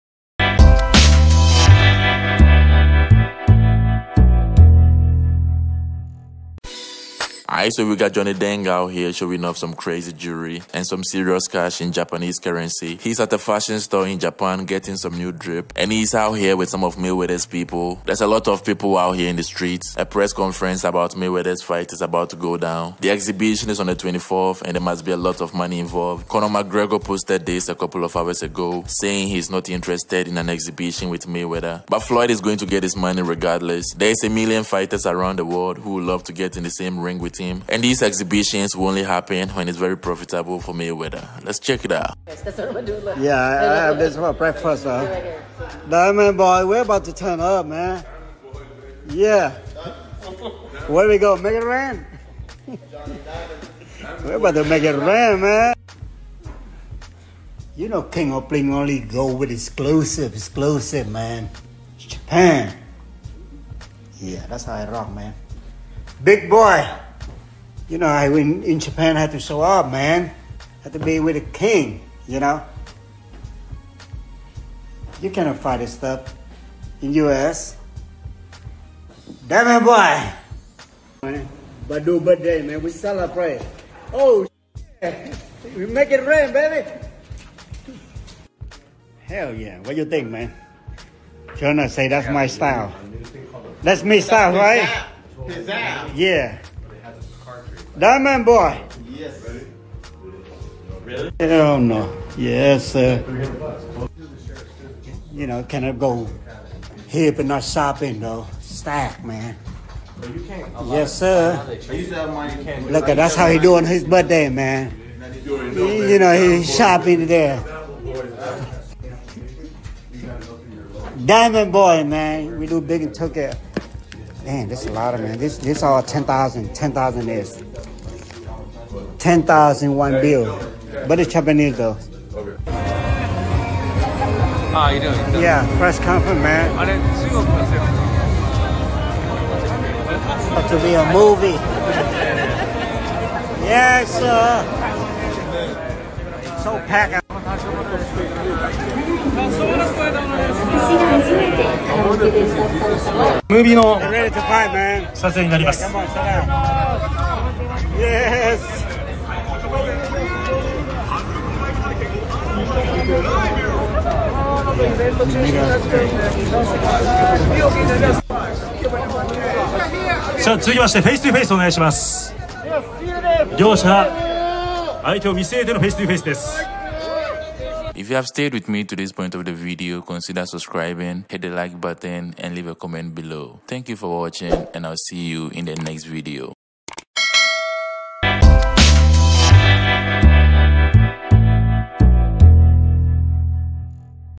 Johnny Dang Flexing Crazy Jewelry And Cash As He Goes Shopping In Japan With Mayweather's Boys
Johnny Dang Flexing Crazy Jewelry And Cash As He Goes Shopping In Japan With Mayweather's Boys, Pure Jewelry.